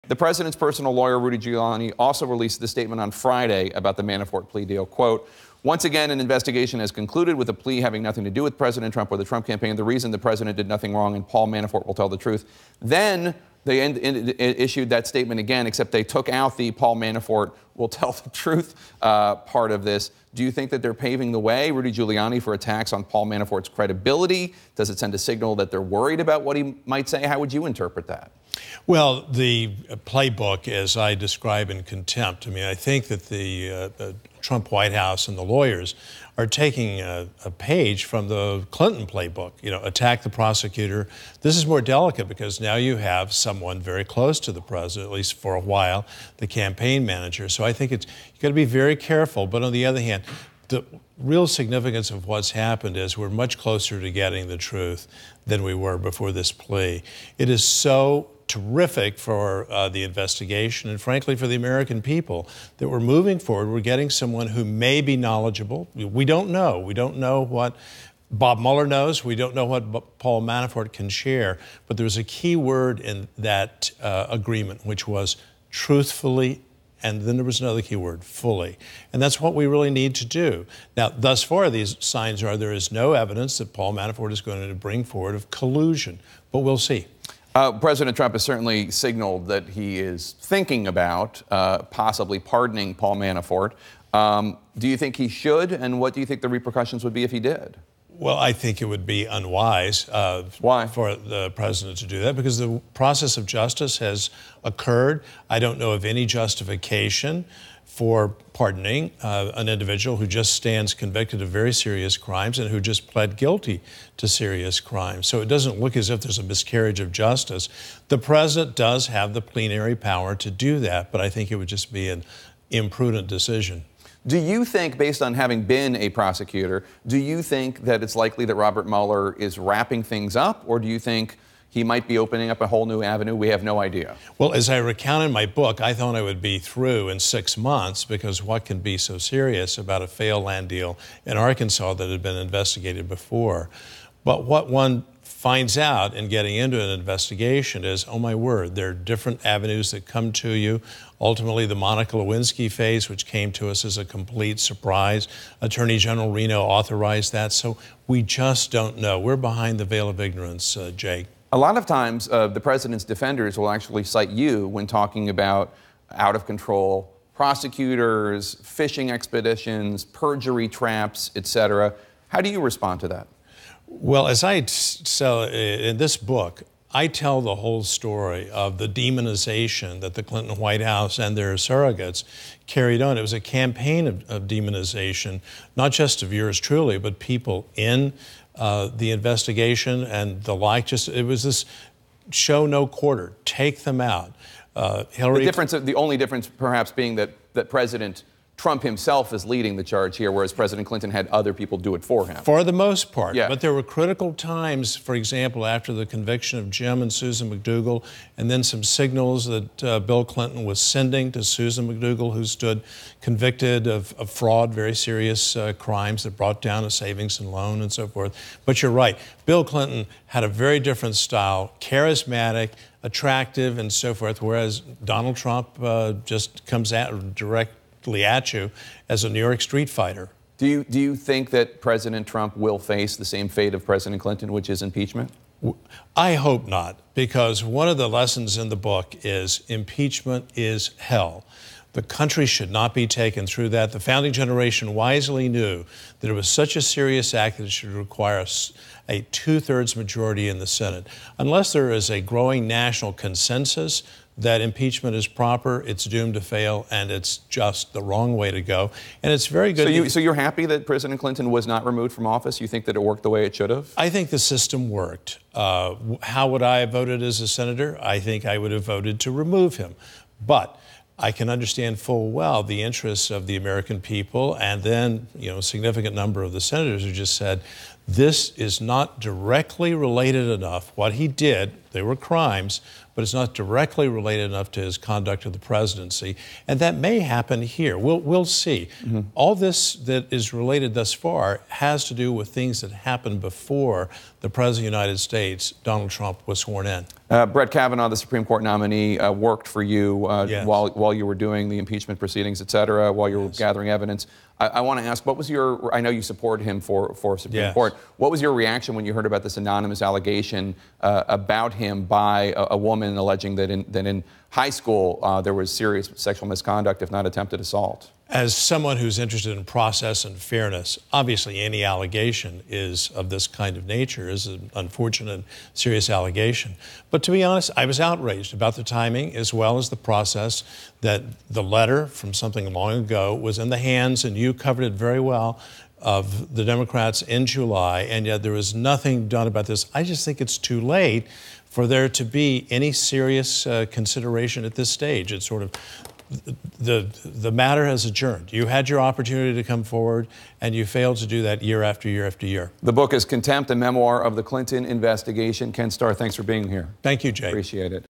Kenneth Starr cautions against rush to impeach Trump
Former special counsel Kenneth Starr drew comparisons between the the investigation of former President Bill Clinton, which he led, and Robert Mueller's investigation of President Donald Trump, but cautioned about the calls to impeach a president.